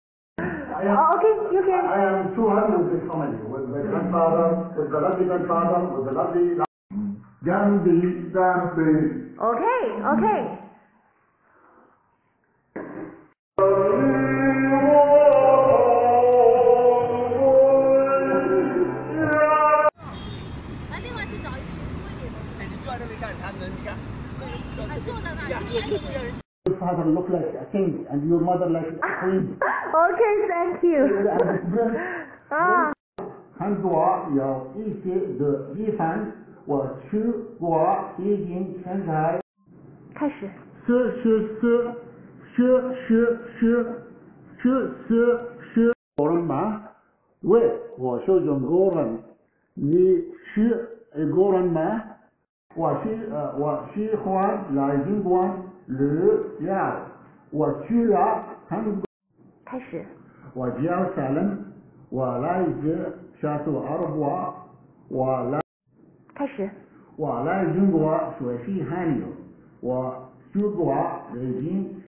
Salem homestay with Cherry's family in Dalian
Salem came from Saudi Arabia. Here the homestay with teacher Cherry's family in Dalian. The video is taken by Wechat on the phone, the size is small.